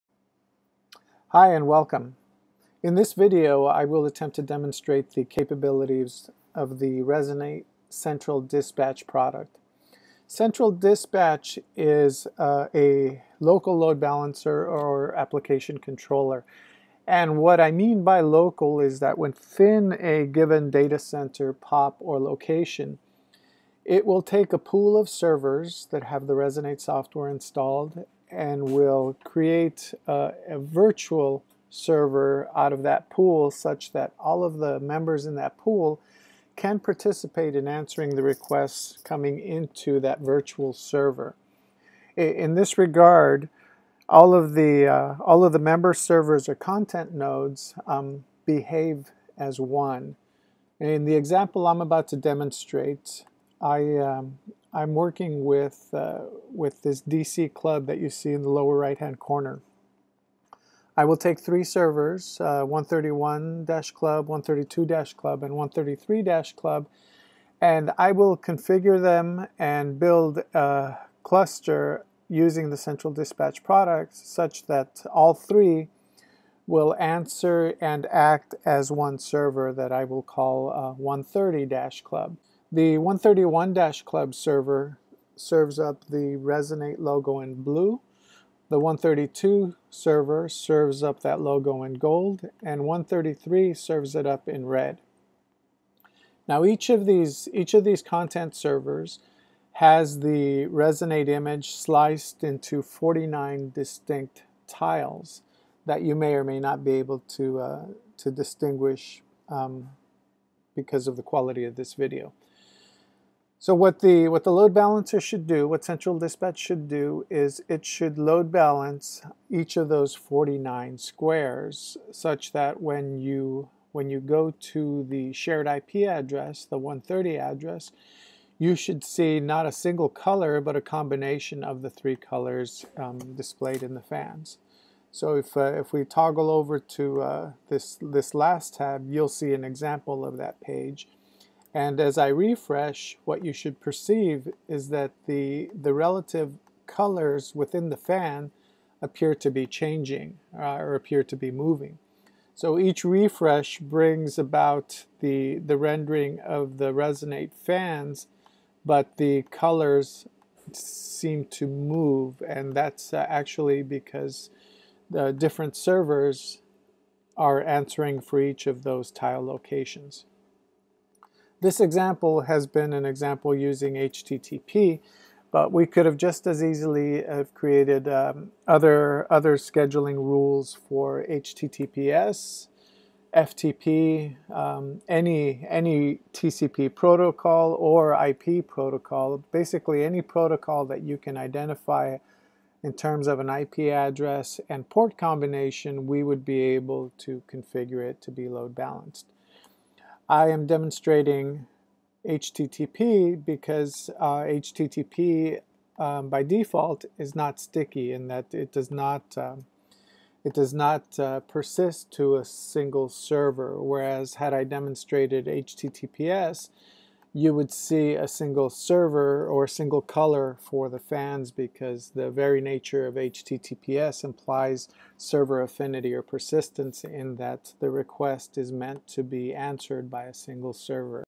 Basic demo of Central Dispatch
This is a basic demo of the Central Dispatch product. It will demonstrate  three content nodes acting as one through the Central Dispatch product.